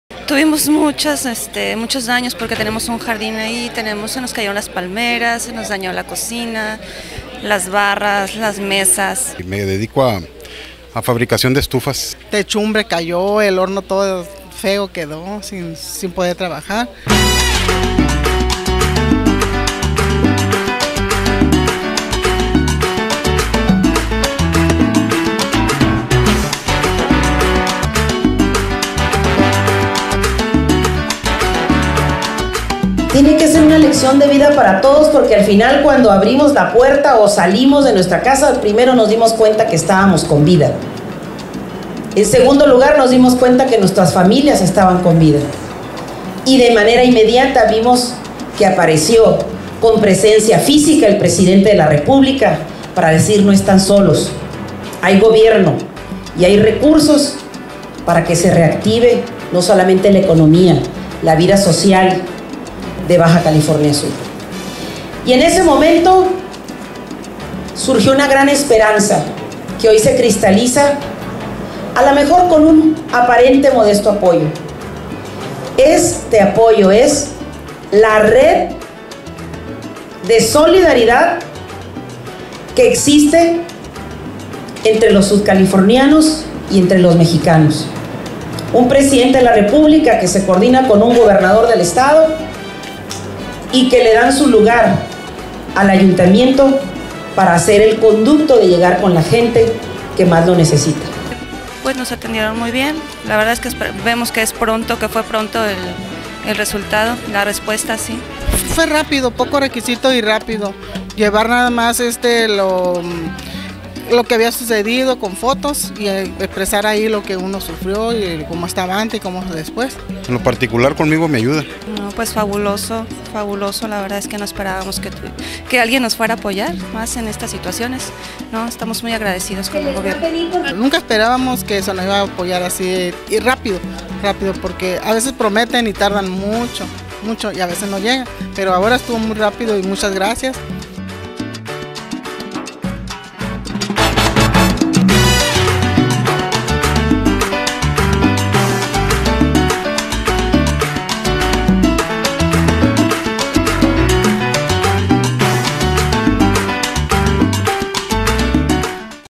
Apoyos para microempresarios paceños afectados por Odile.
Microempresarios ‪#‎paceños‬ cuyos negocios fueron afectados por ‪#‎ODILE‬ recibieron apoyos gestionados por el H. Ayuntamiento de La Paz a través de su presidenta, Esthela Ponce Beltrán, otorgados por la Secretaría de Economía del Gobierno de la República a través del Instituto Nacional del Emprendedor.